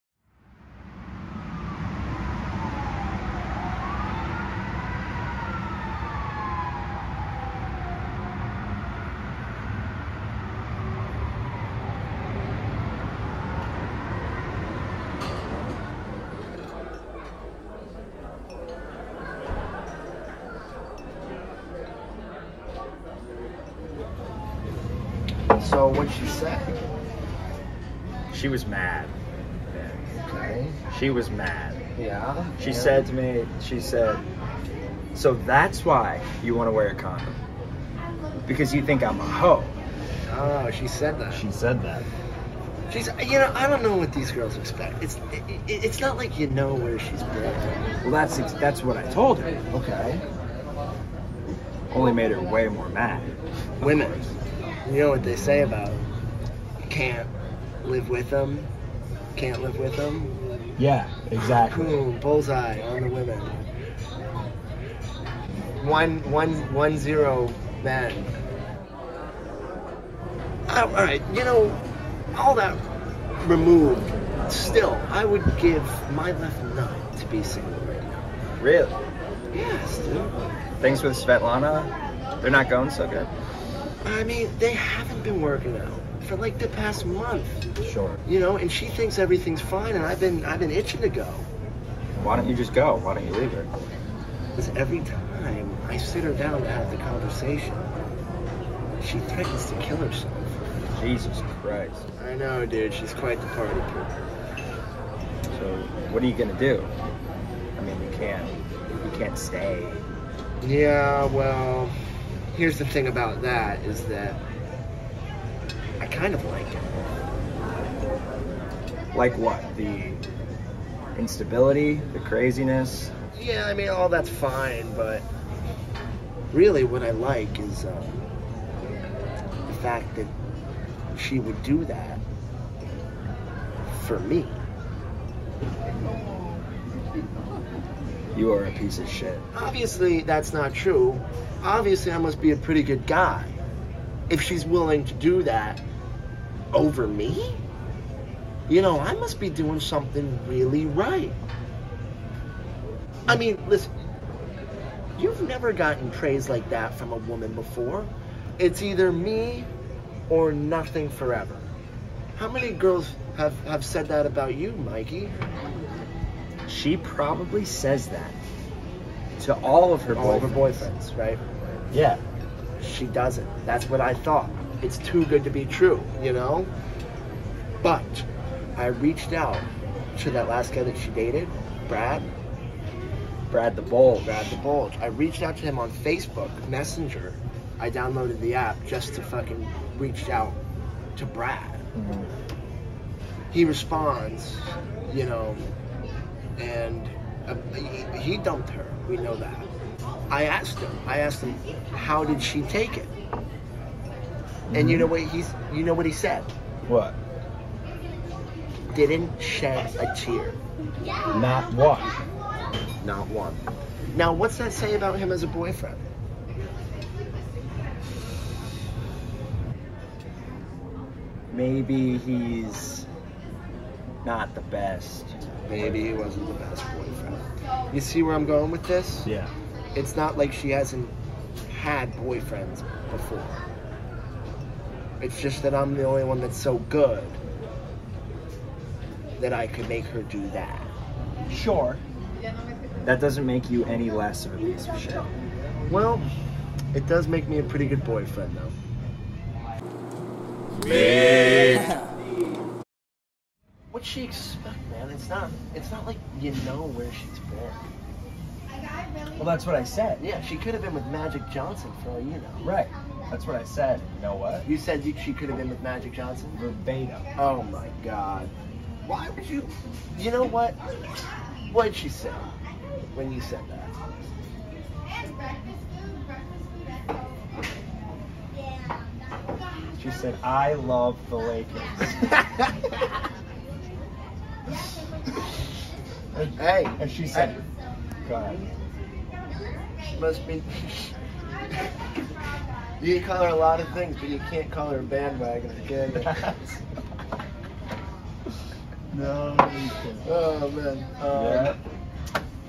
Midnight Coffee: Girlfriend
Ricky and Mikey discuss a compliment worth staying for.